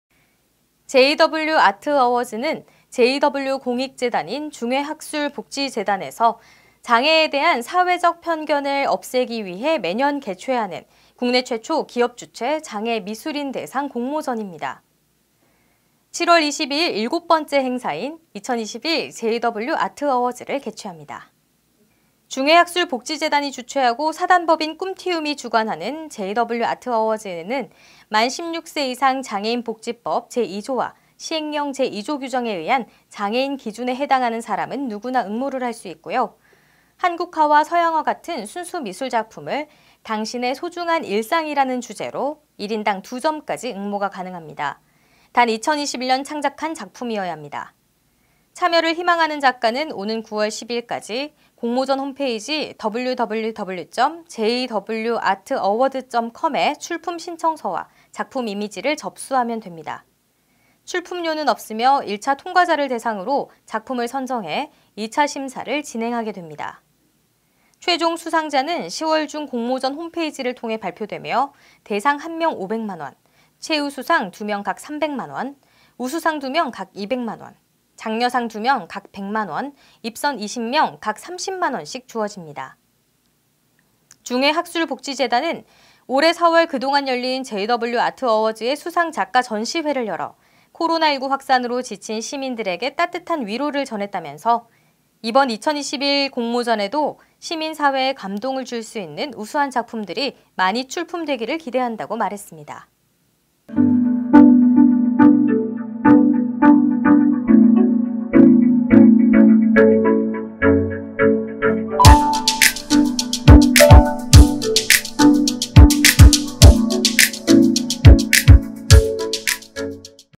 JW그룹, 장애인 미술공모전 ‘2021 JW 아트 어워즈’ 개최
미술공모전#2021 JW 아트 어워즈#장애인#중외학술복지재단#순수미술#